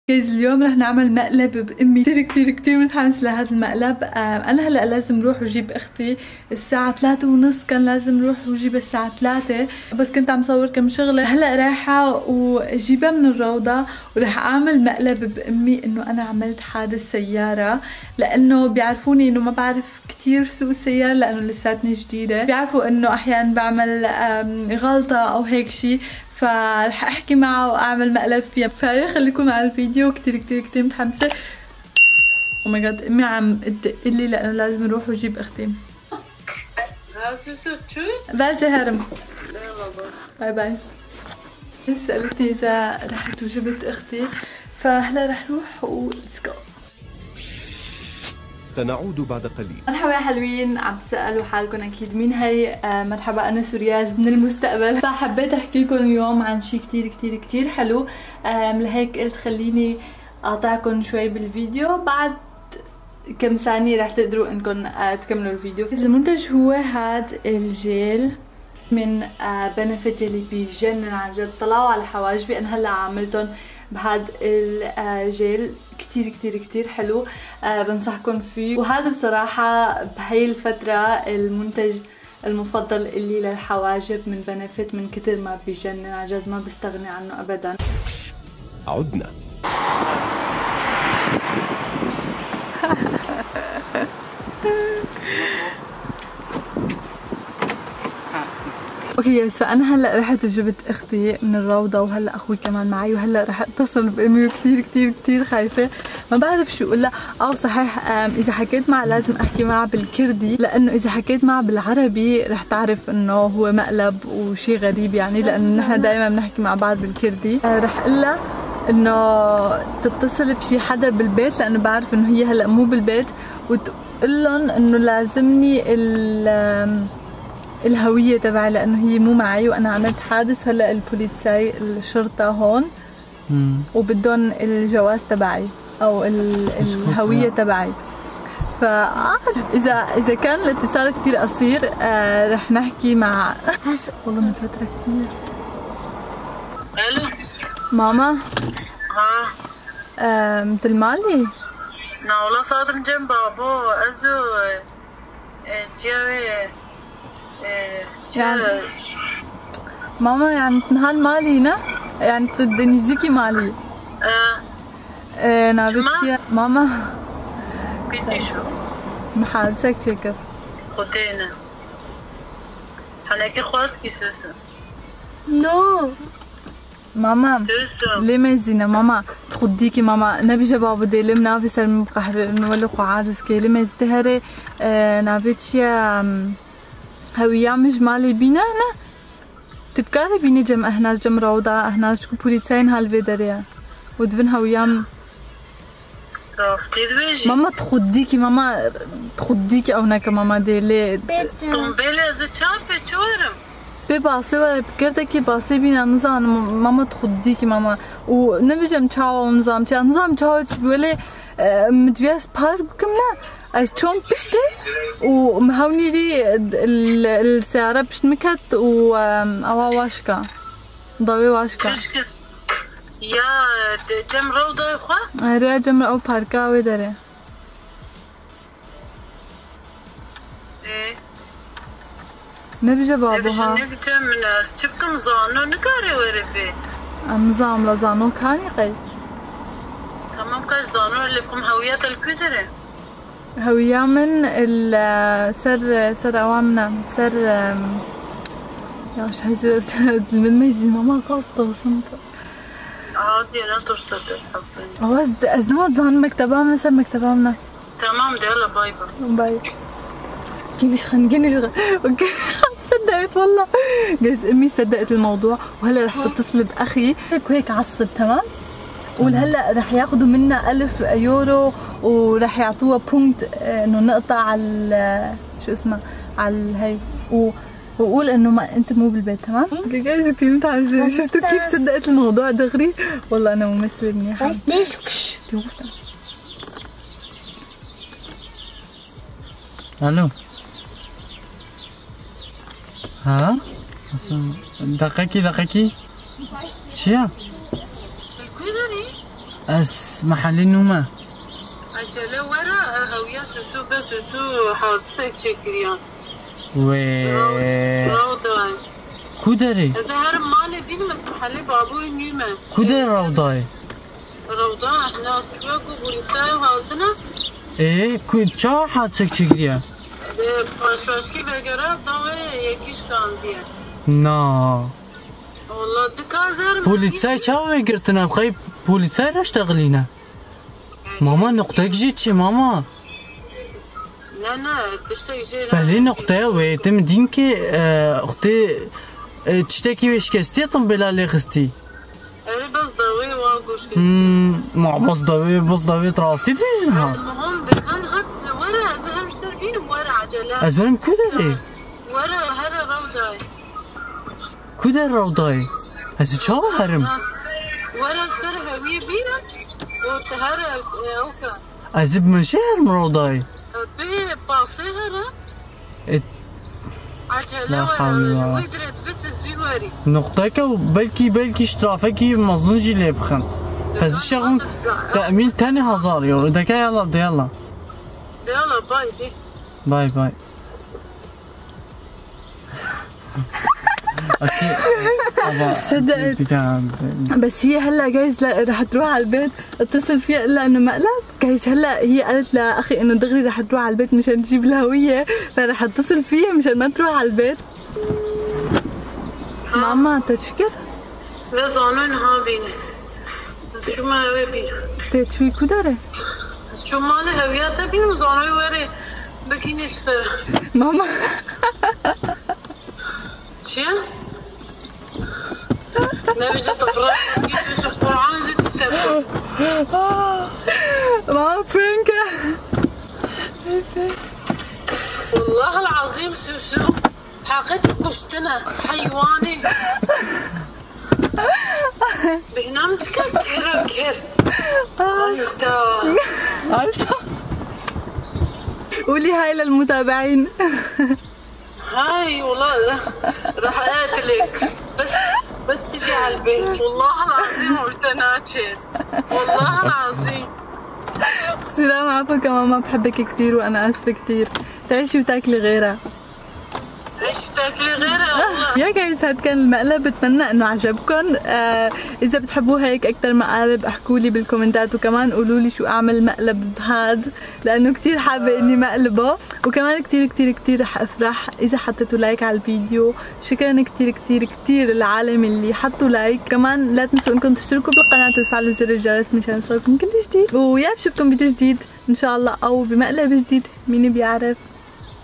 عملت حادث سيارة / شوفوا ردة فعل امي 😱
💞استعملت منتجات بنفت لحواجبي💞 KA-BROW from Benefit